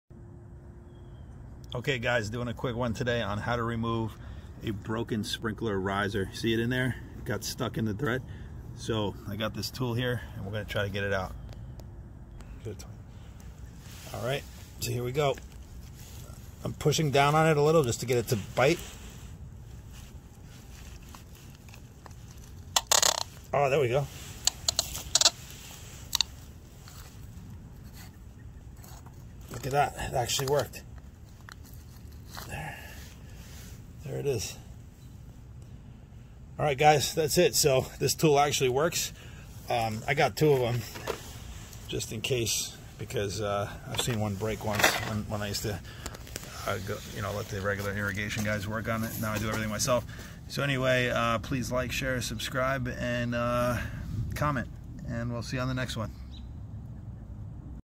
Fix A Broken Sprinkler Riser/Nipple In Under A Minute
In this video we use an Orbitz extractor tool to remove a broken sprinkler head nipple that sits in the elbow.  It is the piece that attaches the sprinkler head to the elbow and these sometimes break in a way that makes them hard to remove.  We make quick work of it here.